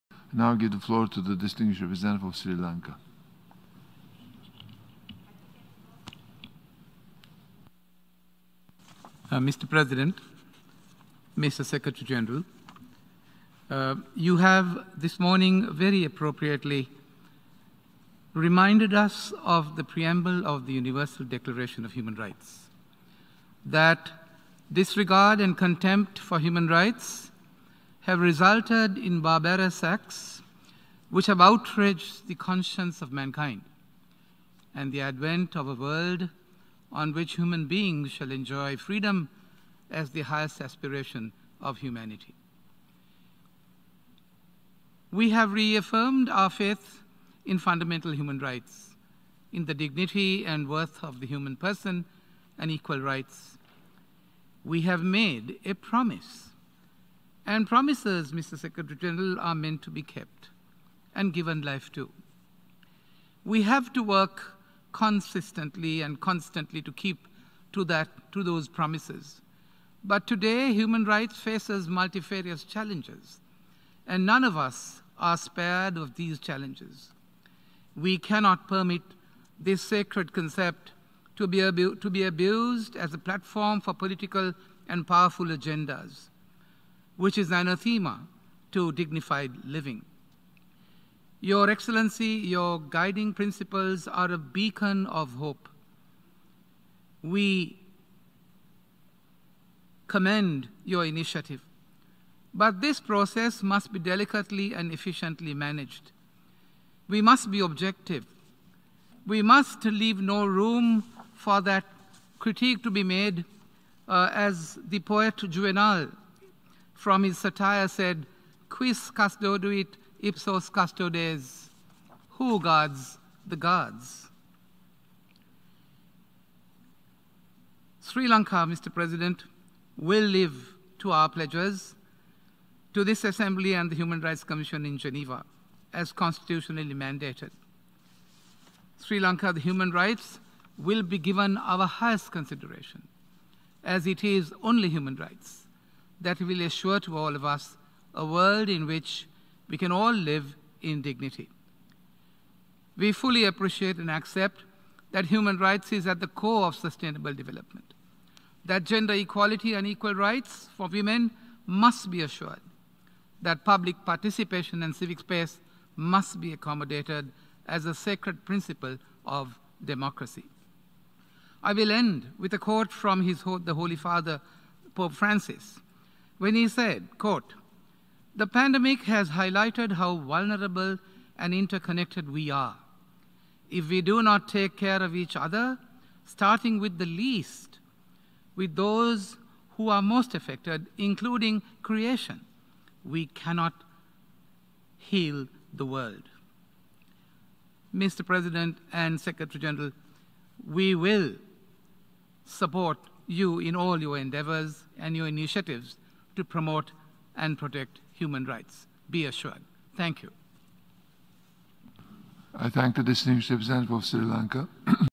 Who guards the guards?
Who guards the guards?  ( Quis custodiet ipsos custodes? Latin phrase found in the work of the Roman poet Juvenal from his Satires )

Statement by H.E. Mohan Peiris, Permanent Representative of Sri Lanka at the General Assembly: Briefing by the Secretary-General on the "Call to Action for Human Rights" - Informal meeting of the plenary, 75th session on 24 February 2021